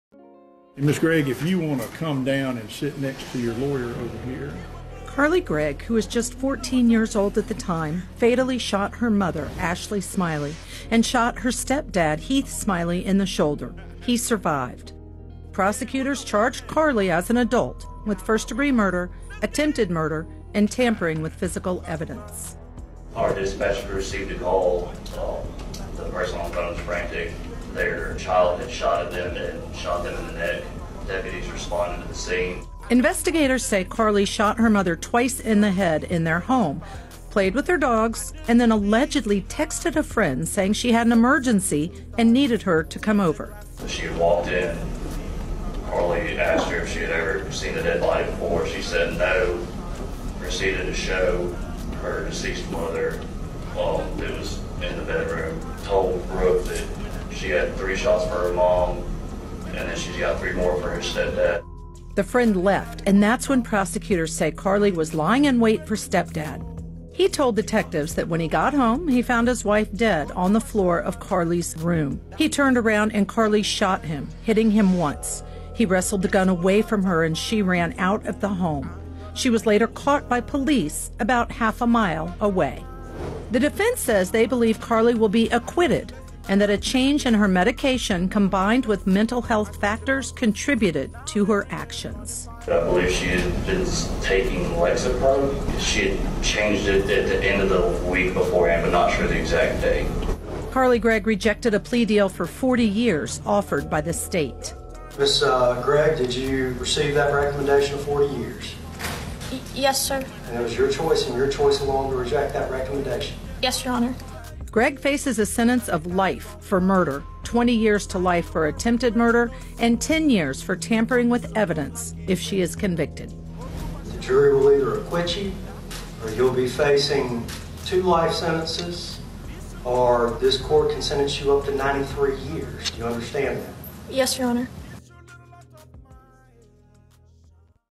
Carly Gregg was 14 years old when she allegedly shot her mother twice in the face
Carly Gregg was 14 years old when she allegedly shot her mother twice in the face and shot her stepfather in the shoulder. She now faces a jury charged as an adult after rejecting a plea deal of 40 years. The teen now faces life in prison if found guilty of murder. CourtTV What do YOU think?